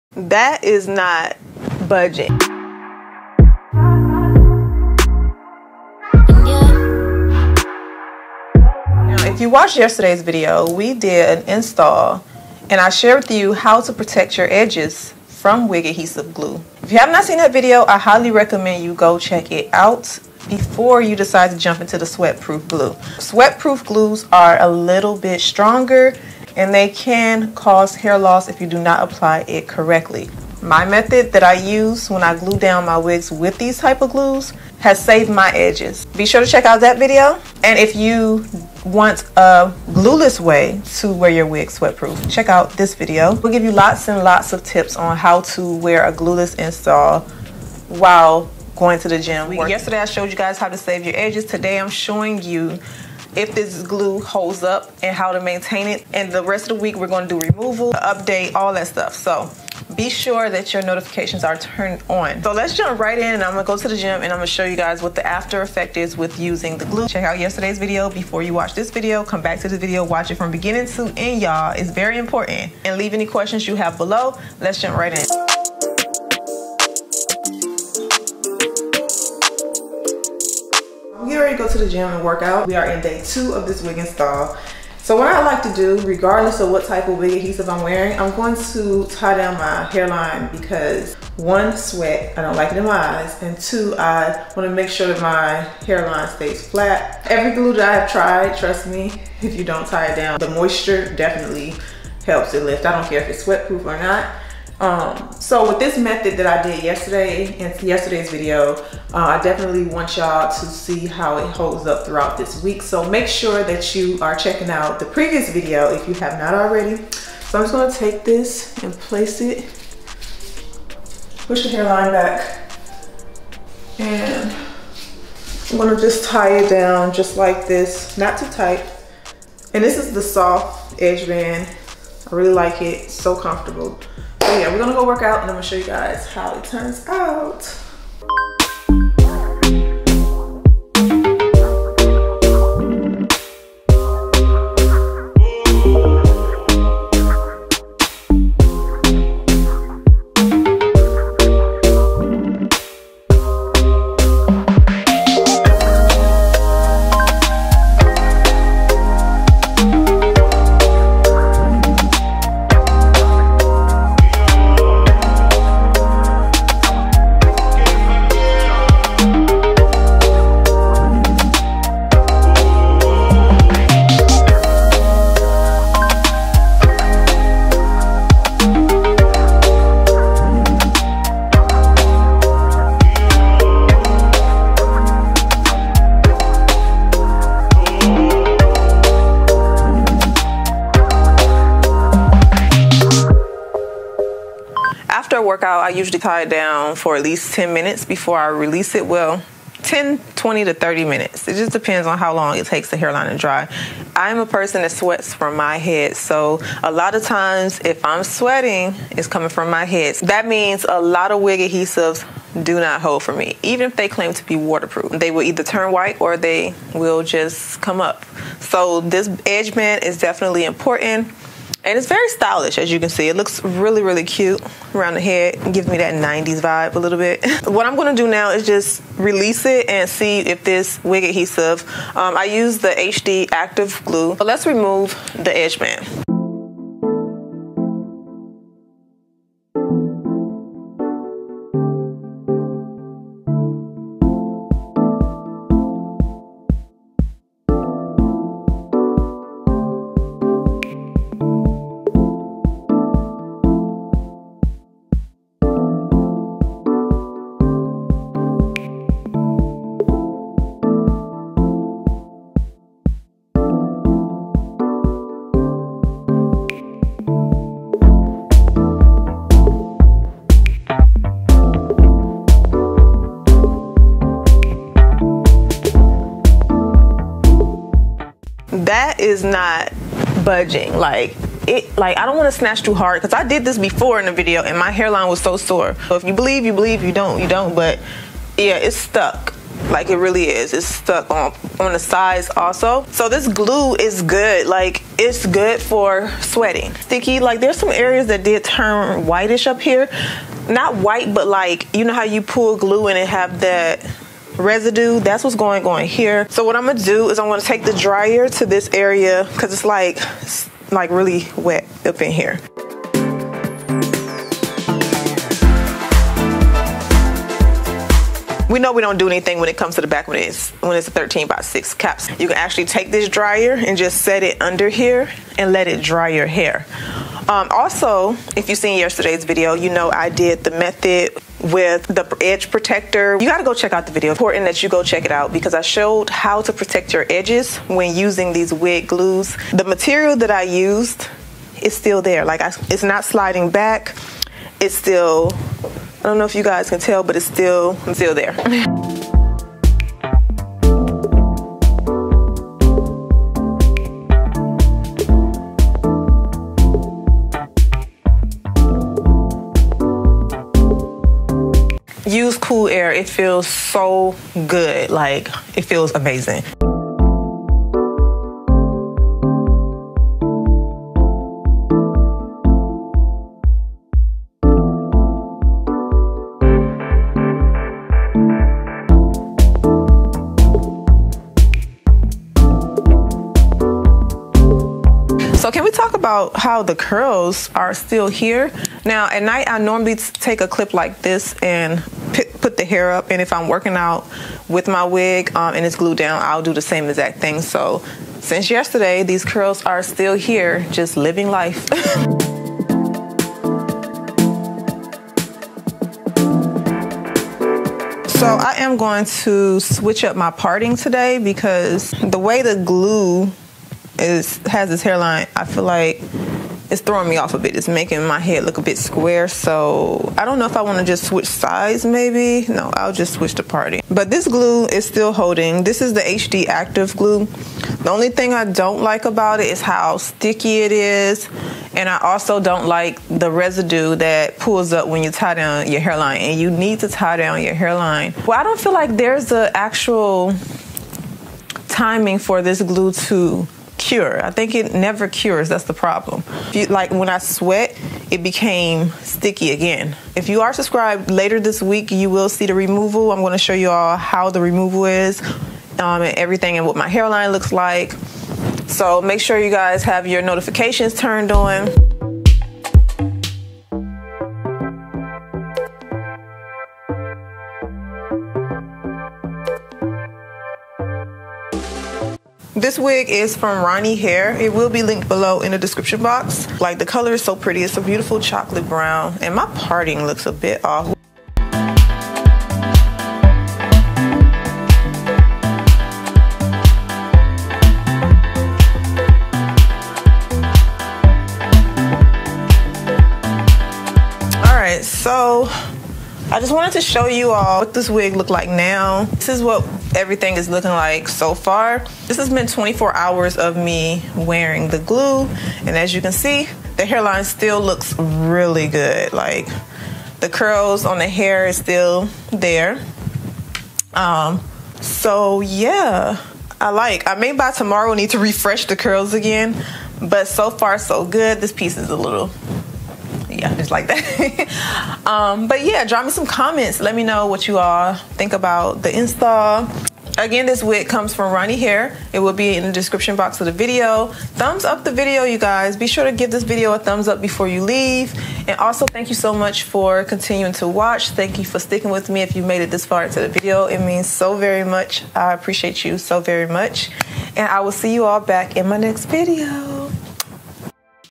🙌🏽 SWEATPROOF HD GLUE: Wig Install That Is SWEAT Proof | HD Active Glue | Ronniehair
Sweat Proof wig glue tested for a workout, swimming, and long wear.

✨ DISCOUNT: Marie
LENGTH: 18 inch
DENSITY:150
DESCRIPTION:13X6 Lace Front Wig